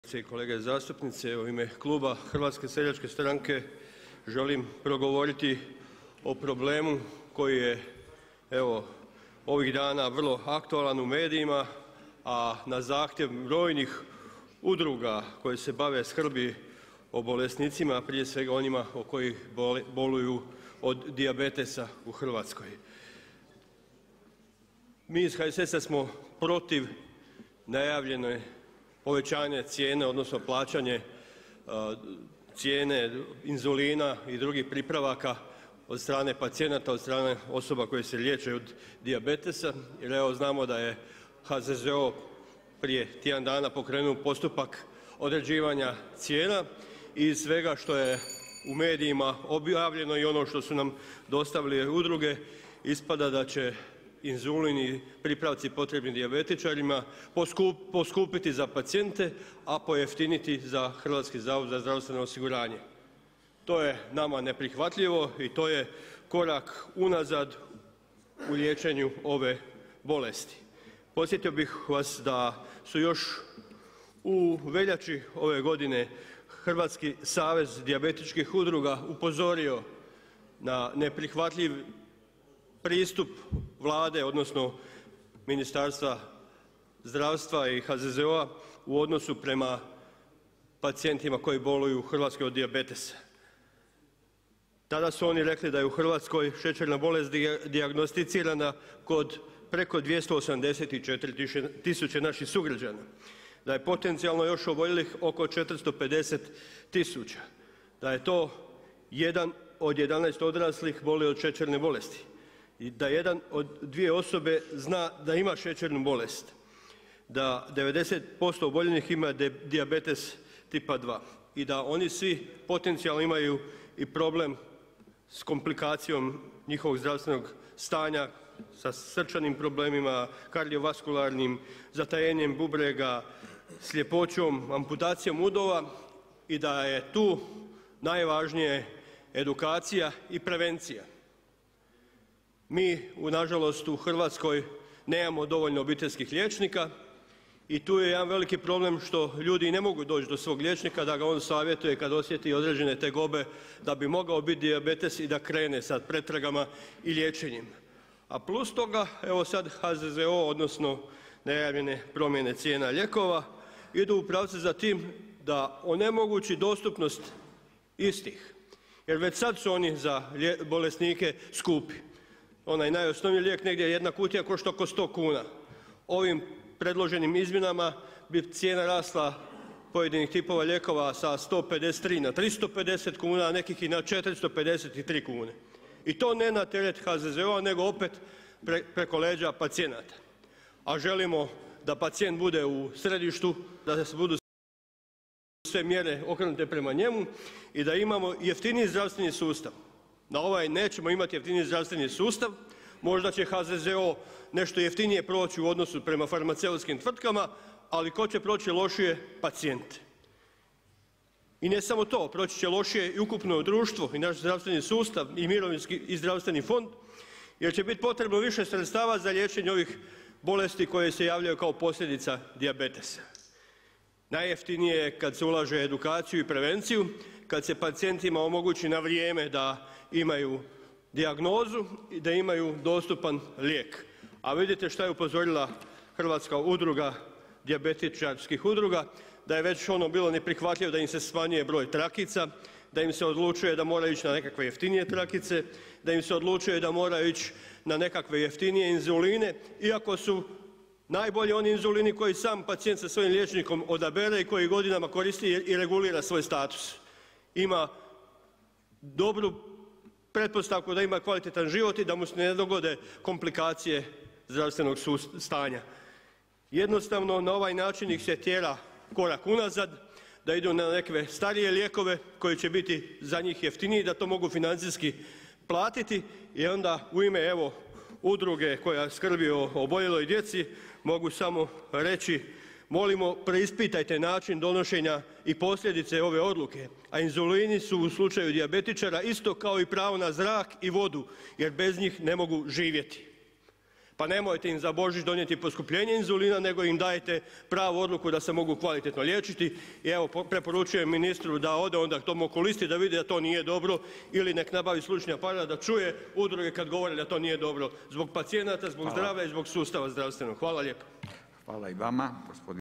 Vlaović: Nemojte dijabetičarima za Božić donijeti poskupljenje inzulina!
Mi iz HSS-a smo protiv toga da se inzulin doplaćuje jer je on dijabetičarima neophodan! Odlukom koja je donijeta na sjednici Upravnog vijeća HZZO-a inzulin će poskupiti za pacijente, a pojeftiniti za HZZO i to je neprihvatljivo.

Tako će se onemogućiti dostupnost neophodnog lijeka, a opet će se sve prelamati na leđima pacijenata. Oboljele od dijabetesa vraća se korak unazad, na starije lijekove koji će biti jeftiniji.

Citirat ćurečenicu iz dopisa koji nam je stigao od jedne udruge koja okuplja djecu oboljelu od dijabetesa i njihove roditelje: 'Molimo preispitajte način donošenja i posljedice ove Odluke, a inzulini su u slučaju dijabetičara isto kao i pravo na zrak i vodu jer bez njega ne mogu živjeti.'

Nemojte im za Božić donijeti poskupljenje inzulina, nego im dajte pravu odluku da se mogu kvalitetno liječiti. Preporučujem ministru da ide okulistu ako ne vidi da to nije dobro ili neka nabavi slušni aparat da čuje udruge i građane kada govore da to nije dobro.